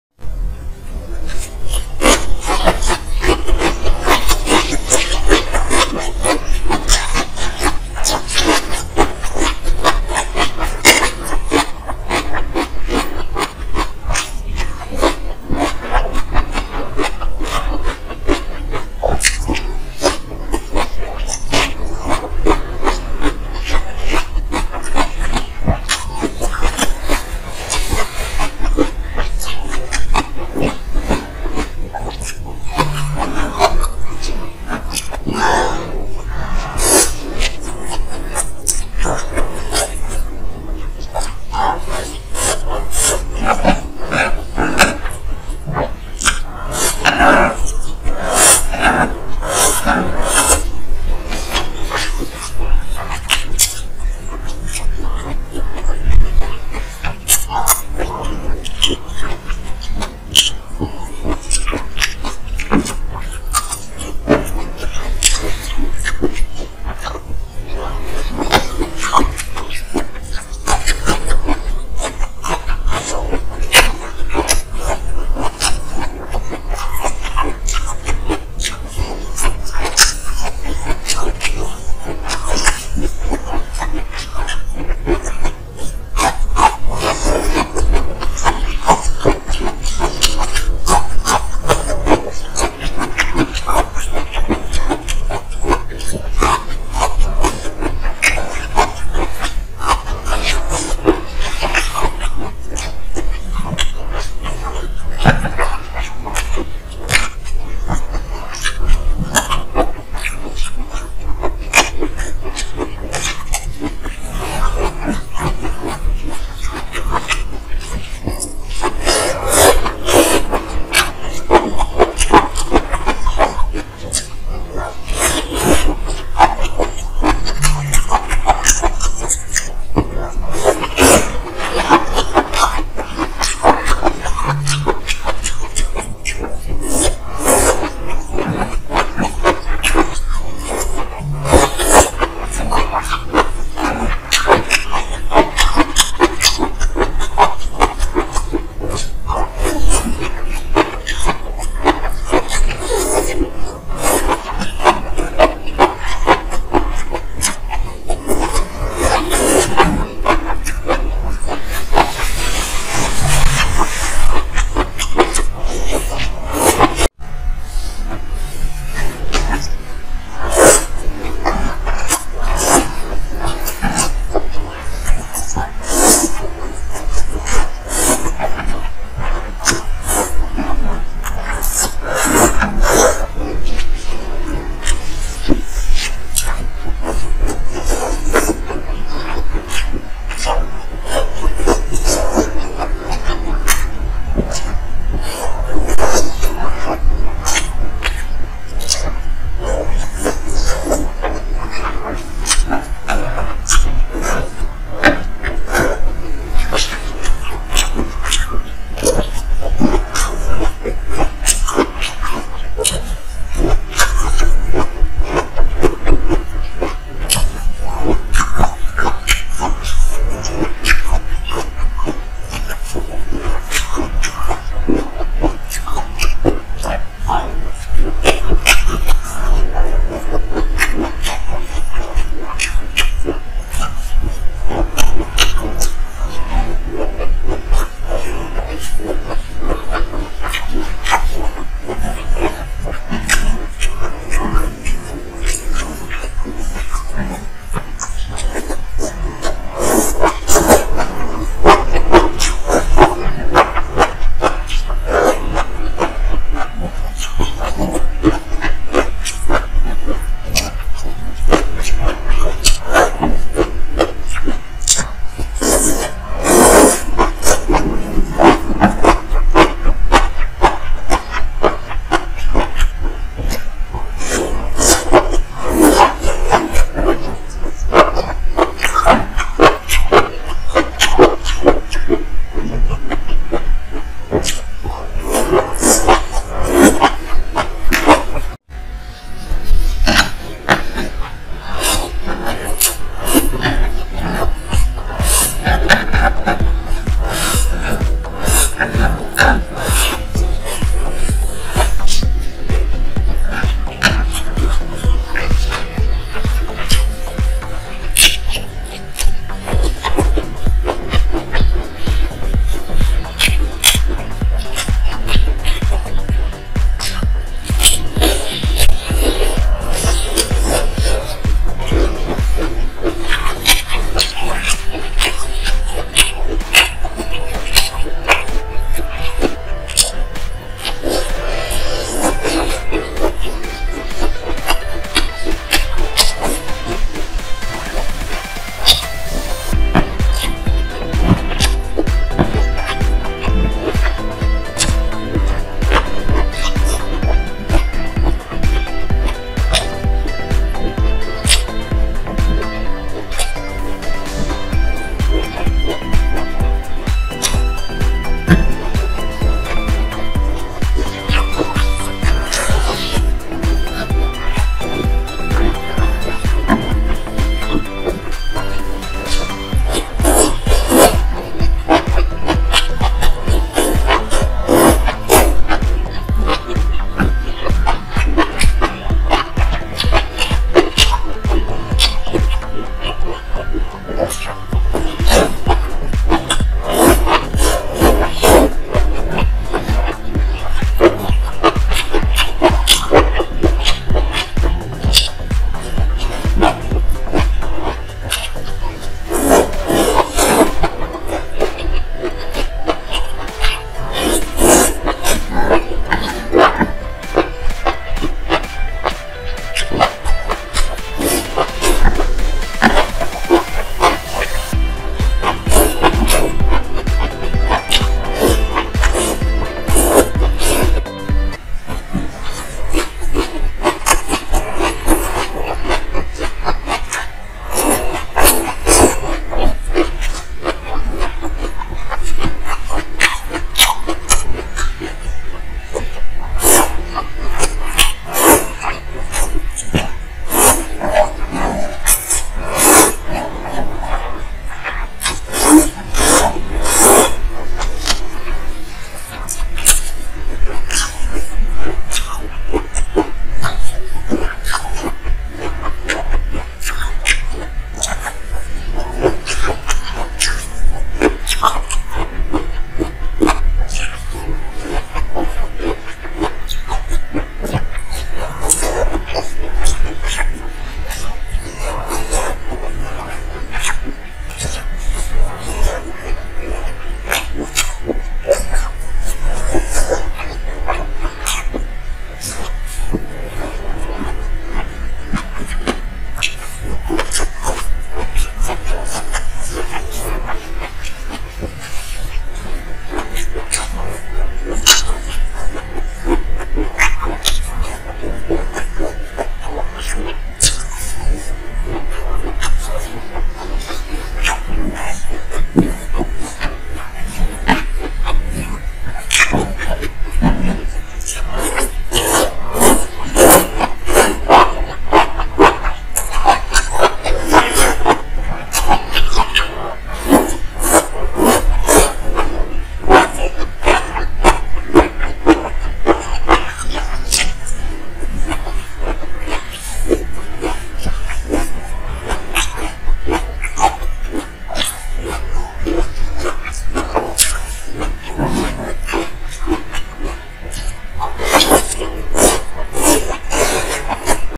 ASMR MUKBANG CHINESE - SPICY NEEDLE MUSHROOM AND SPICY CABBAGE MUKBANG
Source: TikTok Douyin + Kwai
Have fun watching the video and thank you to everyone who has been with us during this time.